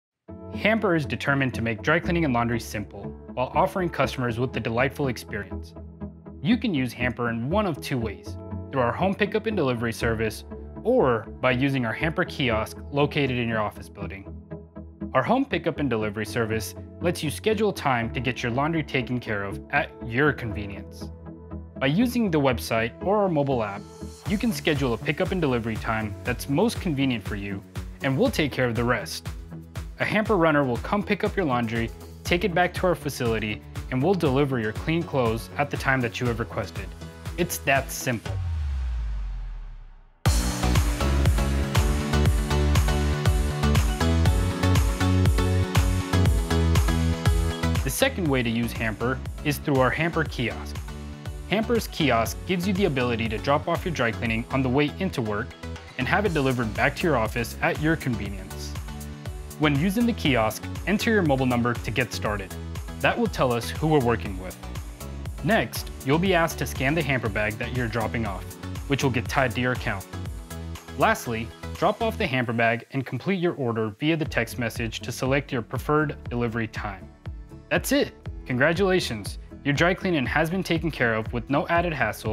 How does Hamper work?
You can use Hamper in 1 of 2 ways: through our home pickup/delivery service or by using our Hamper kiosk.

Our pickup & delivery service lets you schedule time to get dry cleaning and/or laundry taken care of around your schedule!

Our Hamper kiosk lets you drop off your dry cleaning from the convenience of your office building, and we'll deliver your clothes back to you office at the time you've selected!